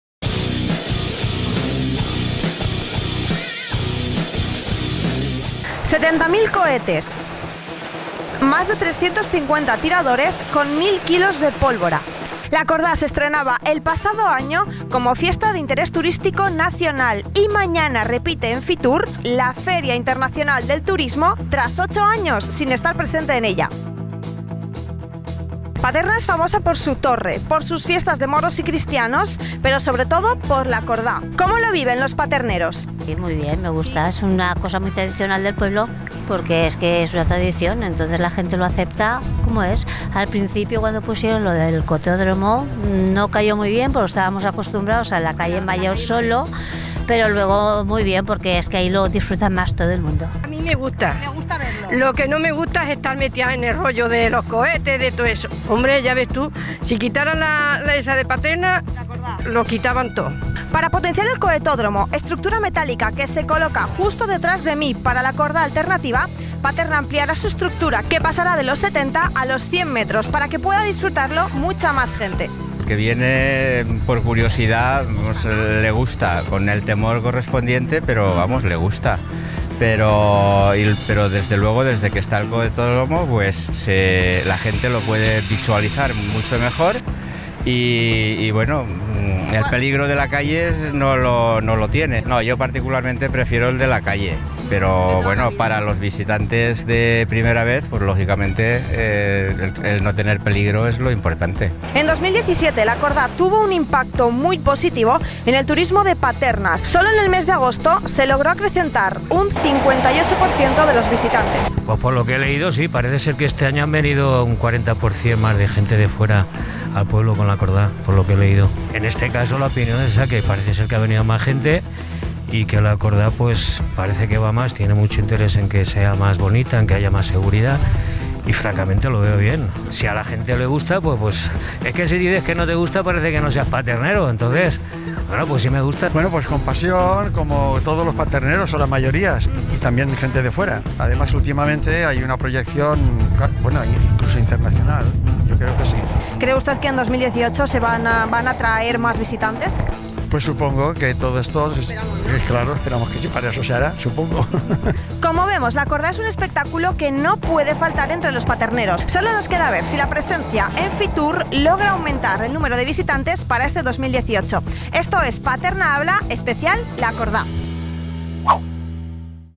Video: Mañana, La Cordà De Paterna​ estará en FITUR, en Madrid
Tras 8 años sin tener presencia en FITUR, mañana la Cordà estará presente en ella, ¿cómo lo ven los vecinos de Paterna?

No te pierdas ninguno de nuestros Paterna Habla, ¡nuestro contenido está hecho por y para ti!